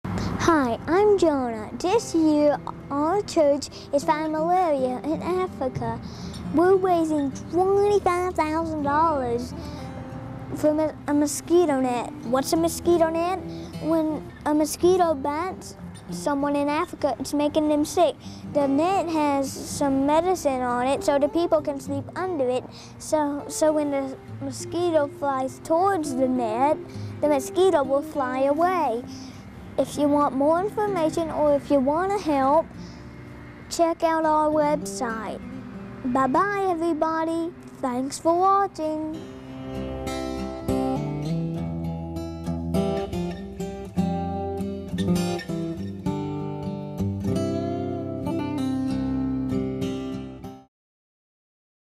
Nothing But Nets
Jonah makes a commercial for the "Nothing But Nets" campaign.